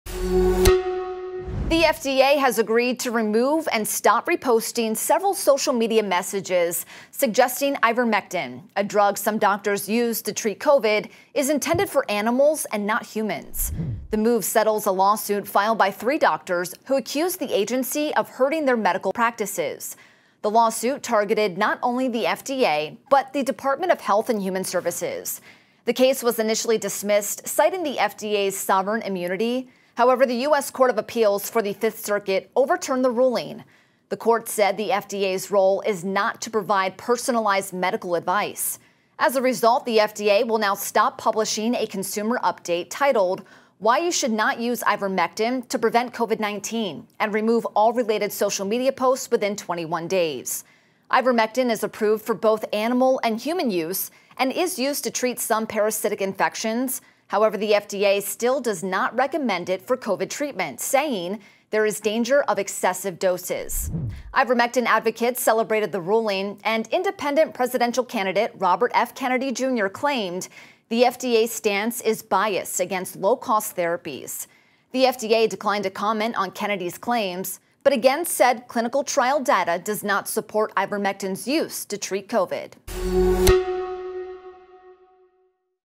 FDA agrees to take down Ivermectin posts in legal settlement with doctors
The Food and Drug Administration has reportedly agreed to remove and stop reposting several social media messages suggesting that Ivermectin — a drug used by some doctors to treat COVID-19 — is intended for animals and not humans. The move settles a lawsuit by three doctors who accused the FDA of hurting their medical practices.